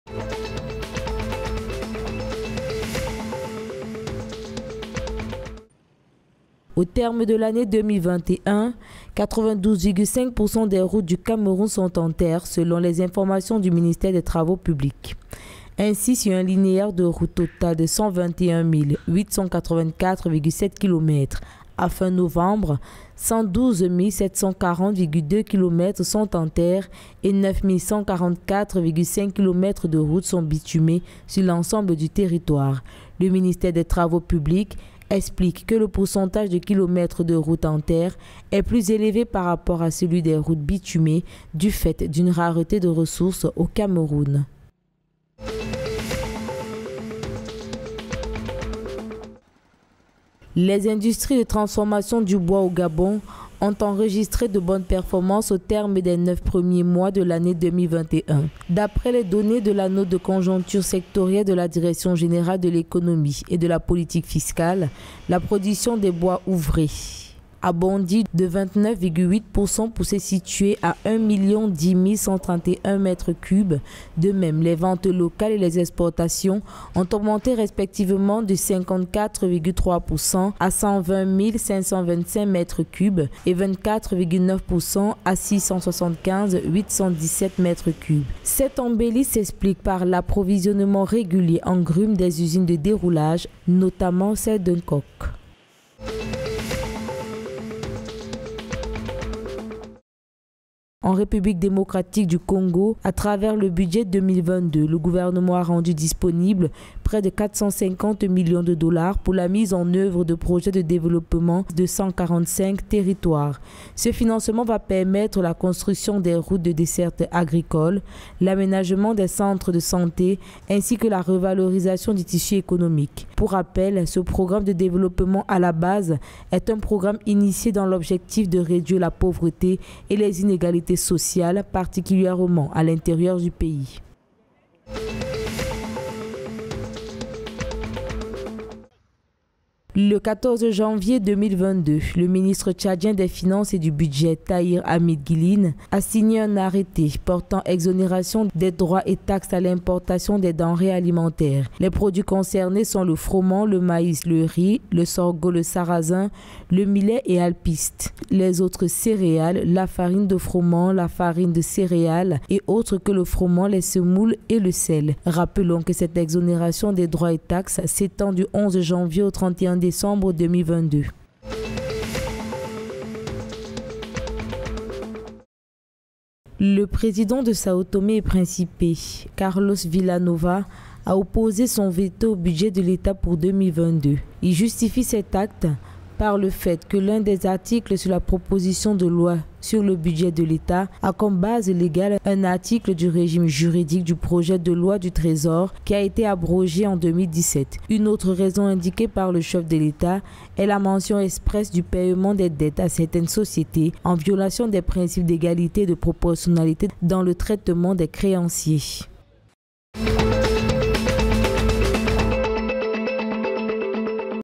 Le journal de l'Afrique Centrale du samedi 15 janvier 2022
Sao Tomé et Principe : le chef de l’Etat Carlos Vila Nova s’oppose au budget général de l'État pour 2022
Tchad : denrées alimentaires exonérées des droits et taxes à l'importation en 2022
RD Congo : 450 millions USD pour le projet de développement des 145 territoires
Gabon : l’industrie du bois renoue avec ses performances d’avant la Covid
Cameroun : 92,5 % des infrastructures  routières en phase de finalisation